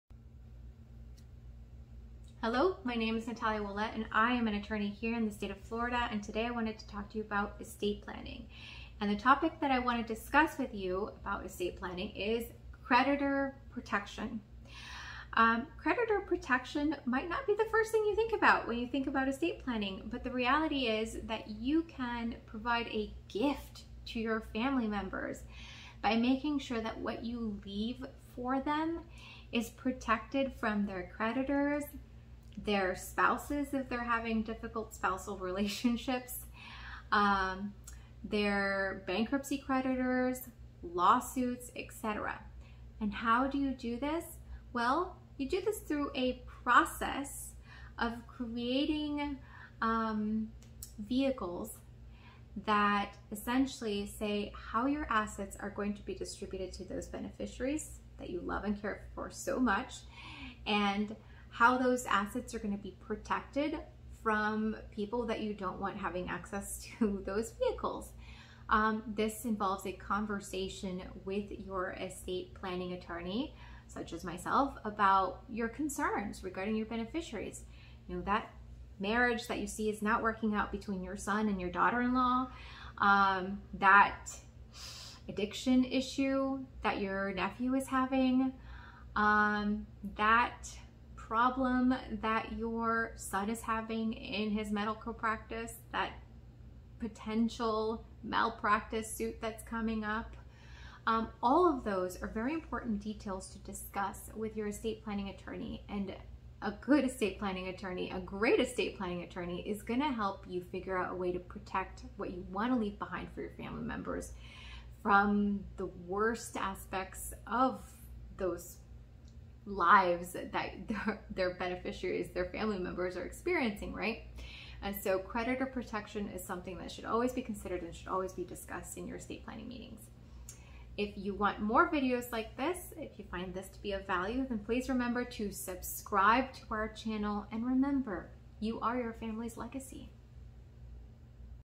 Estate Planning: Creditor Protection
Did you know that you can give your family the gift of creditor protection through proper estate planning? Watch today's video to find out more.

Mention this video for a chance to get this session at no cost to you!